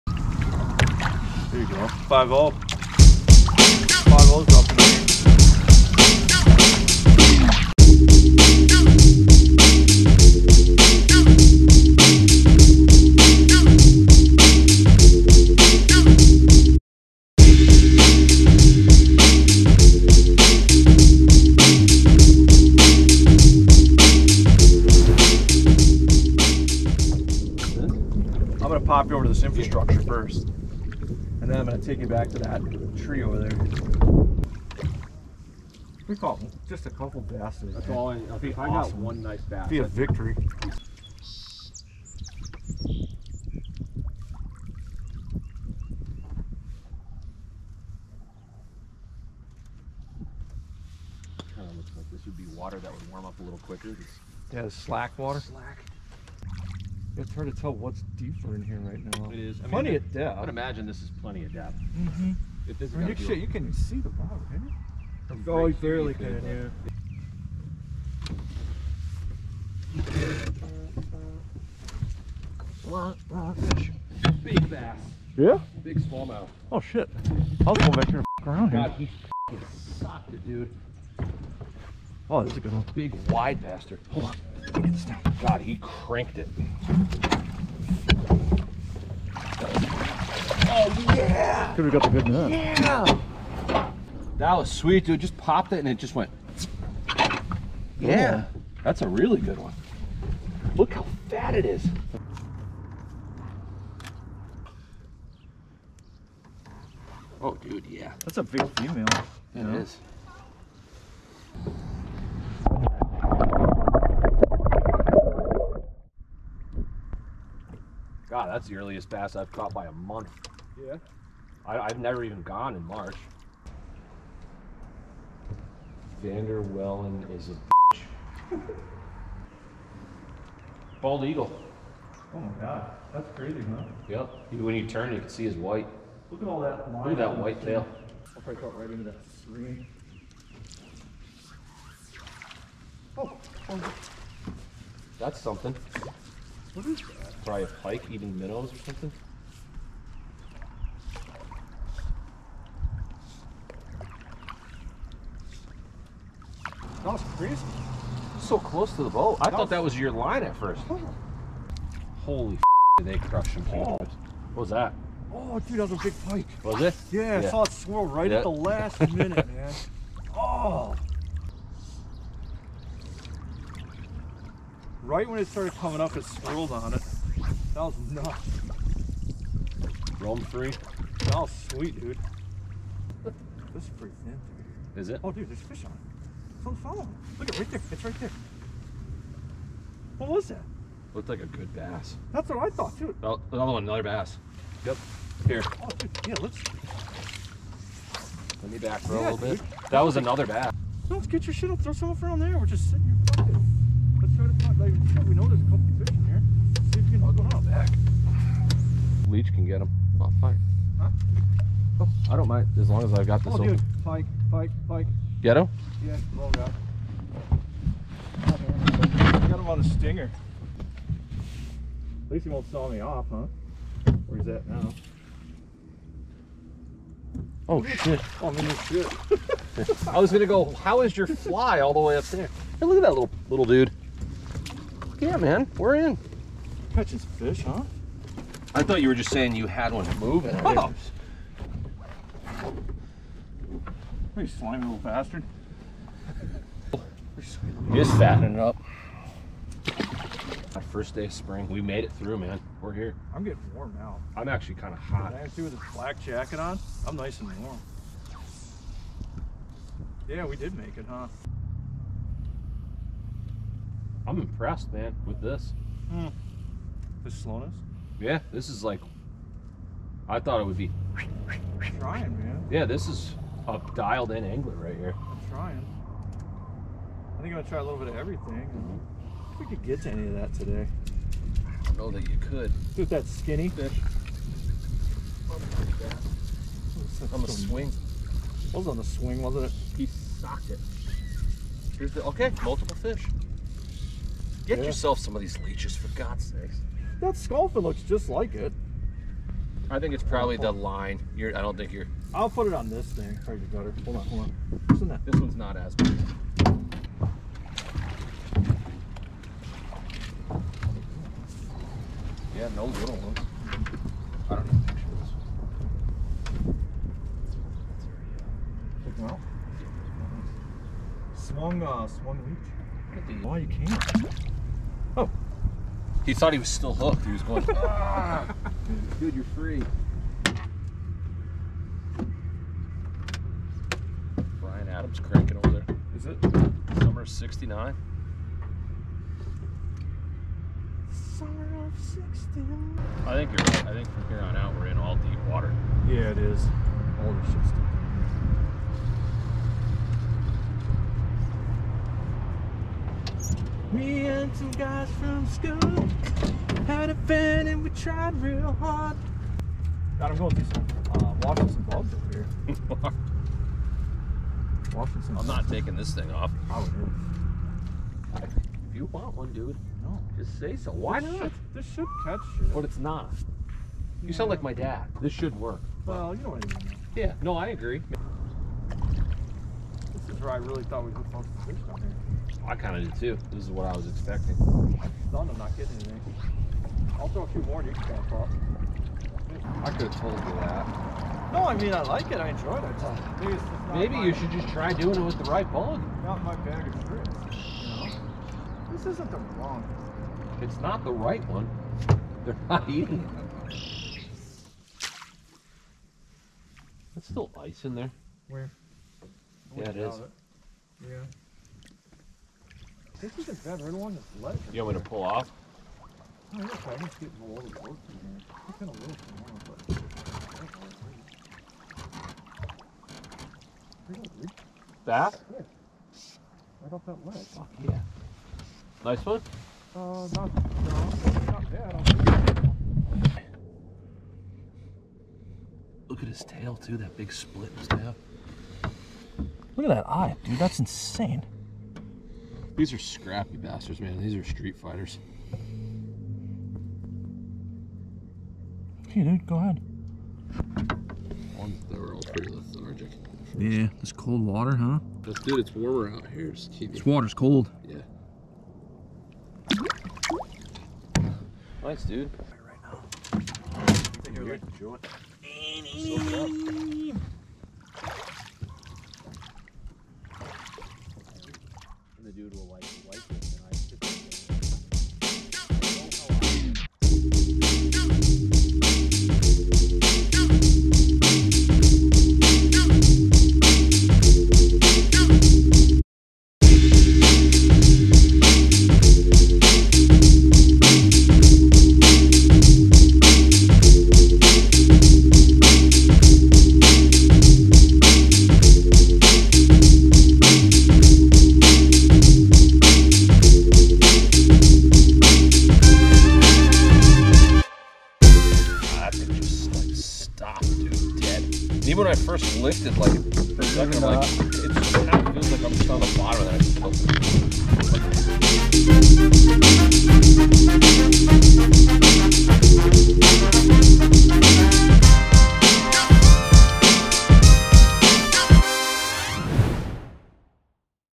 Early Season Spring Smallmouth Fishing for FAT Bass - Michigan
This last year we decided to exit the steelhead scene a little earlier than normal and try our luck on some smallmouth bass.  With the water temperatures being very cold we mostly stripped leech patterns on the bottom early in the day and swam streamers as the temps rose.  Wasn't fast and furious action, but we didn't expect to be.  Even so we found a few players.